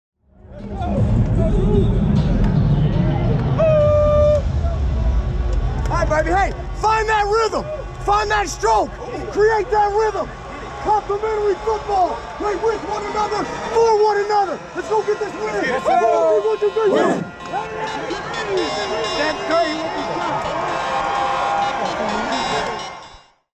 Go 360° Inside Drew Brees' Pregame Huddle: Saints vs. Buccaneers - 9/9/18
Step into the pregame huddle with Drew Brees and the New Orleans Saints at Mercedes-Benz Superdome on September 9, 2018.